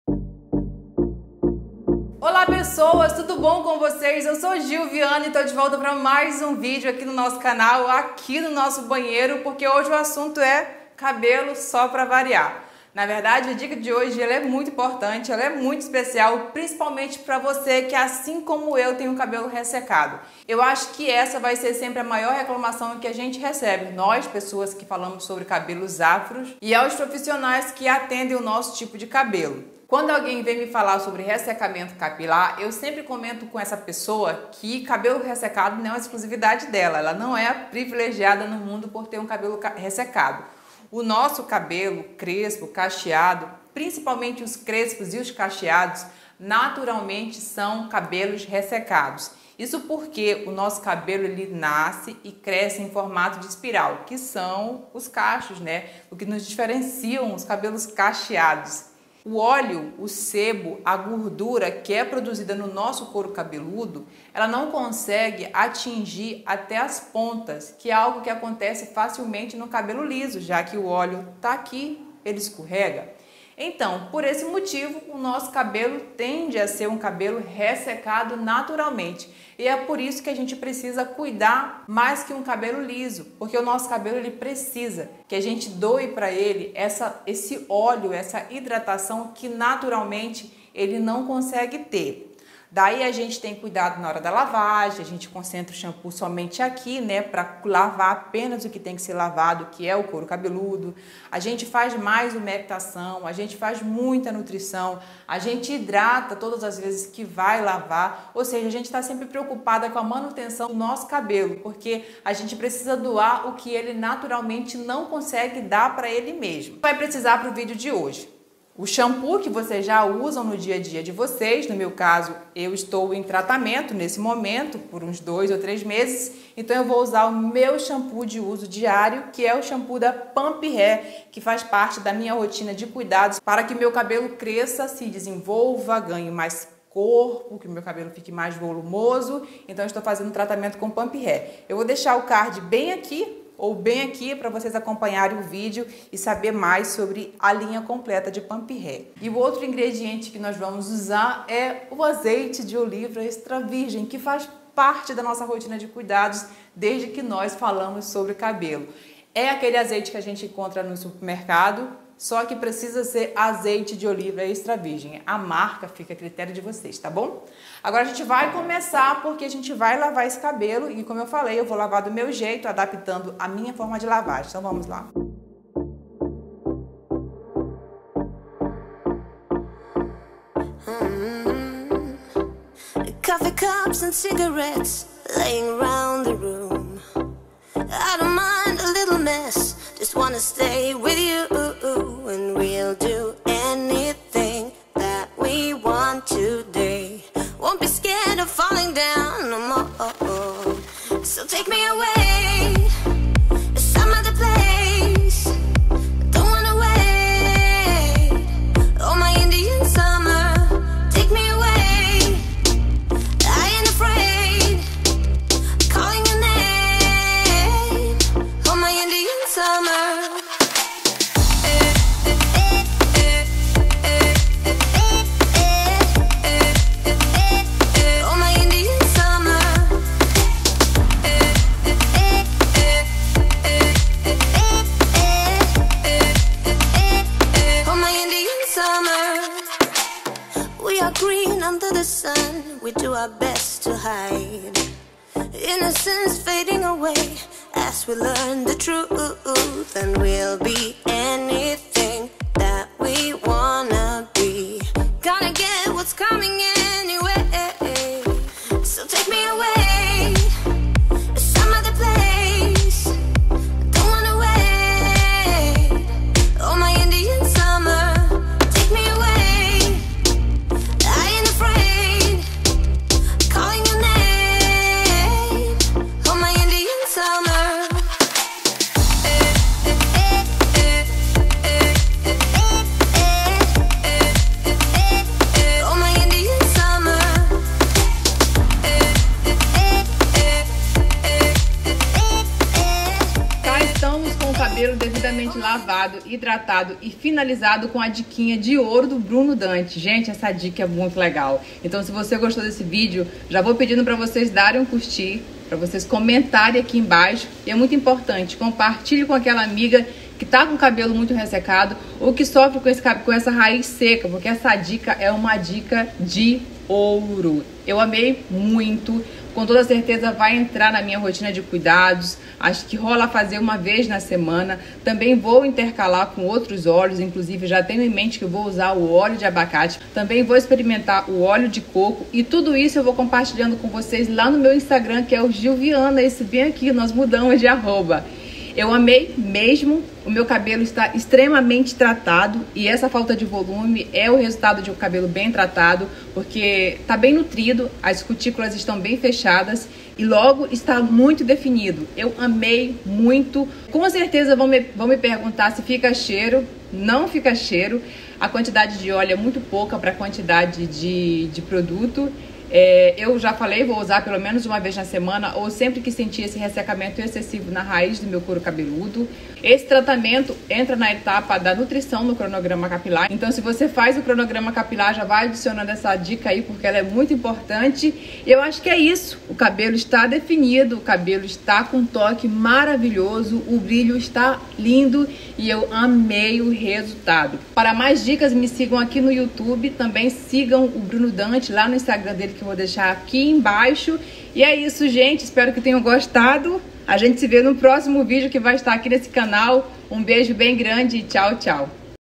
A DICA MAIS SIMPLES DO MUNDO PARA TRATAR CABELOS SECOS E RESSECADOS | por GILL VIANNA
Dica boa é dica compartilhada e essa aprendi com o melhor quando o assunto é cabelo afro, @brunodantte

Se gostou desse conteúdo não deixe de curtir, comentar, compartilhar com as amigas e salvar para vê de novo 💕

Me encontrem também no Instagram